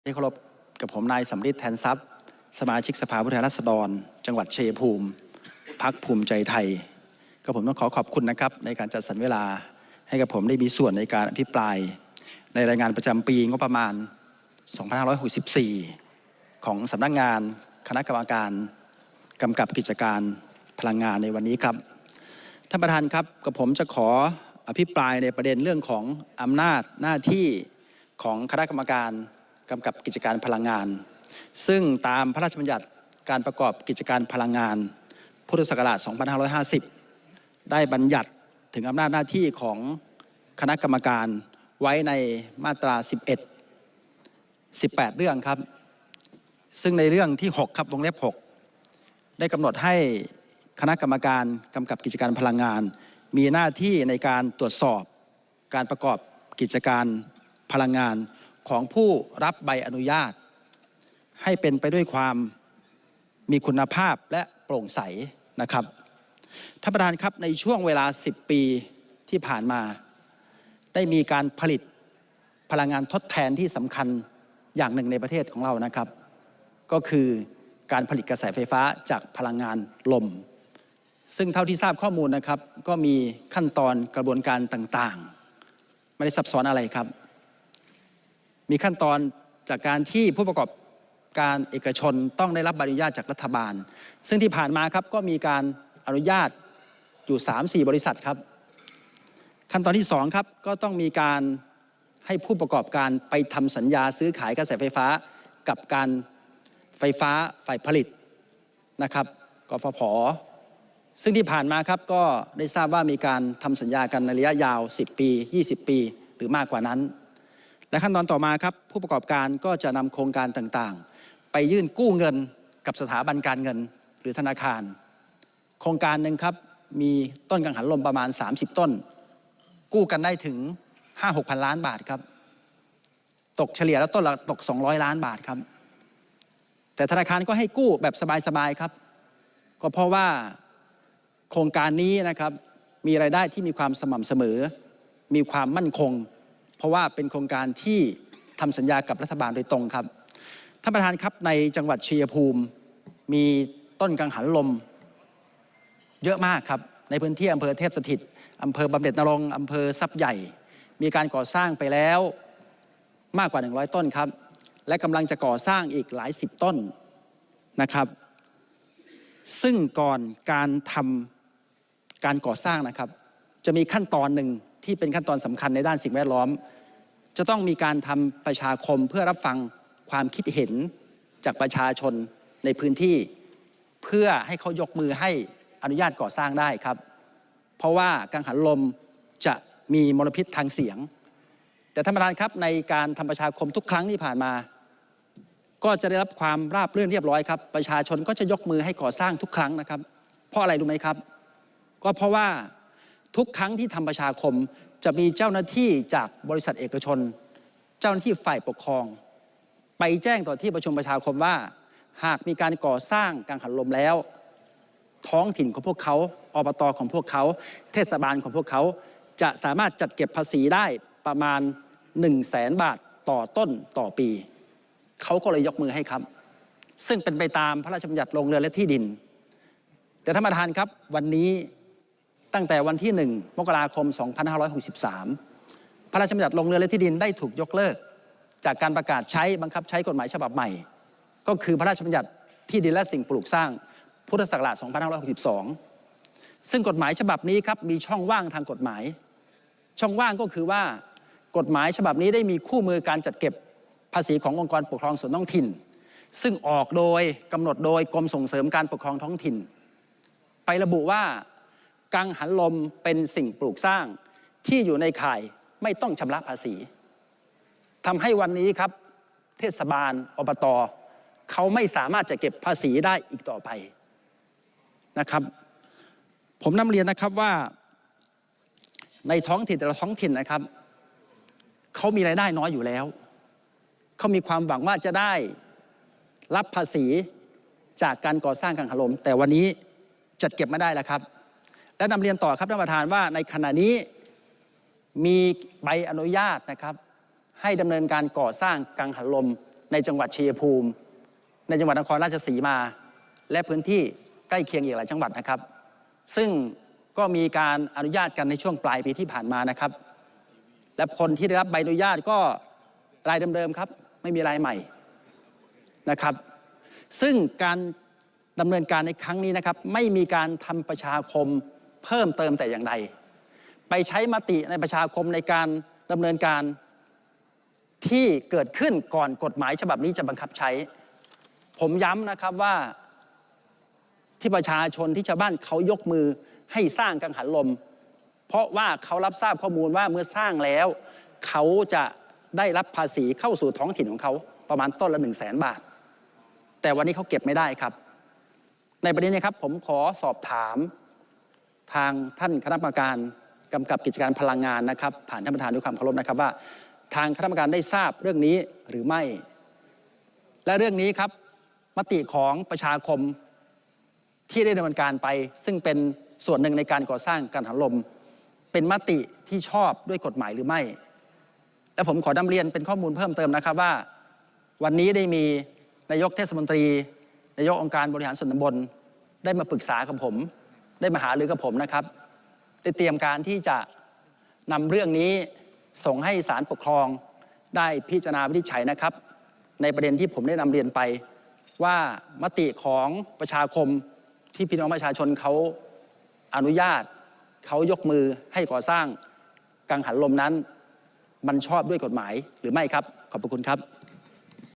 สัมฤทธิ์ แทนทรัพย์
อภิปรายในการประชุมสภาผู้แทนราษฎรชุดที่26 ปีที่1 ครั้งที่11 (สมัยสามัญประจำปีครั้งที่หนึ่ง)
วันพฤหัสบดีที่ 30 สิงหาคม 2566
🔴อภิปราย..รับทราบรายงานประจำปีงบประมาณ พ.ศ.2564 ของคณะกรรมการกำกับกิจการพลังงาน และสำนักงานคณะกรรมการกำกับกิจการพลังงาน